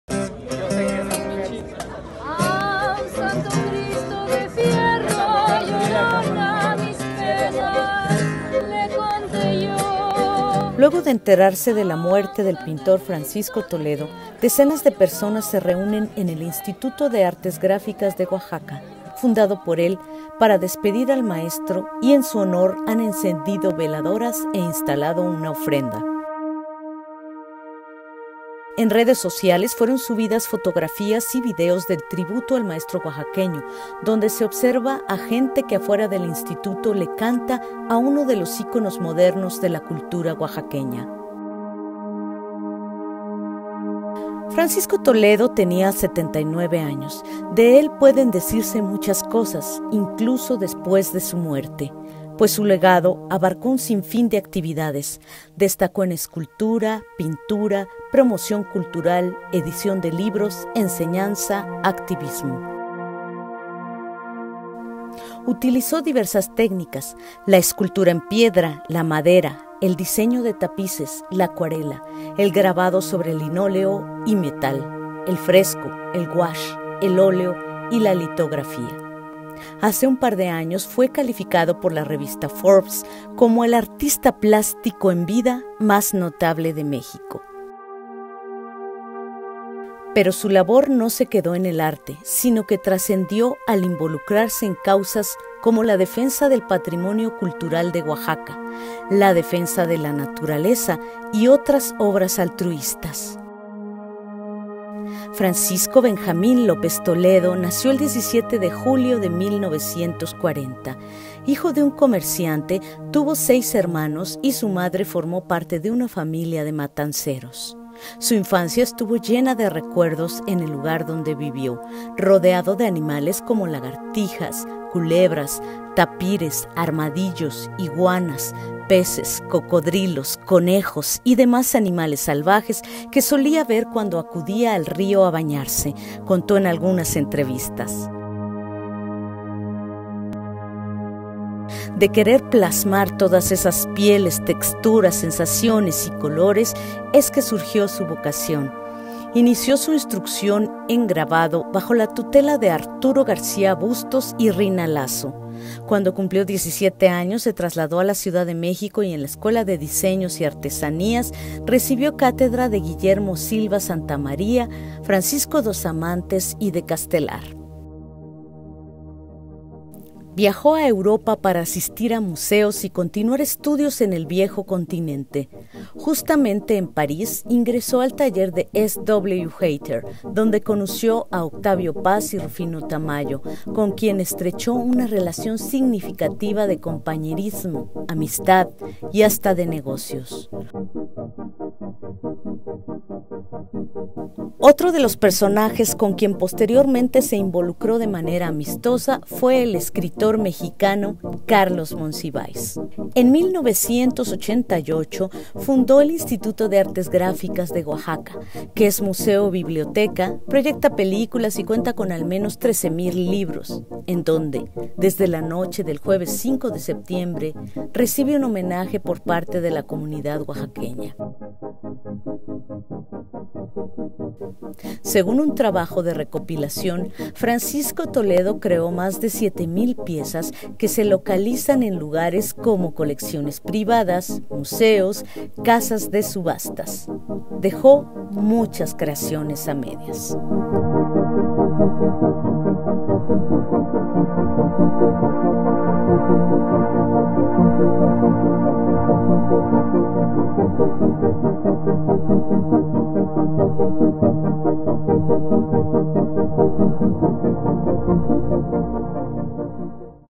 Ofrendas y cantos en honor a Francisco Toledo en Oaxaca
Luego de enterarse de la muerte del pintor Francisco Toledo, decenas de personas se reúnen en el Instituto de Artes Gráficas de Oaxaca.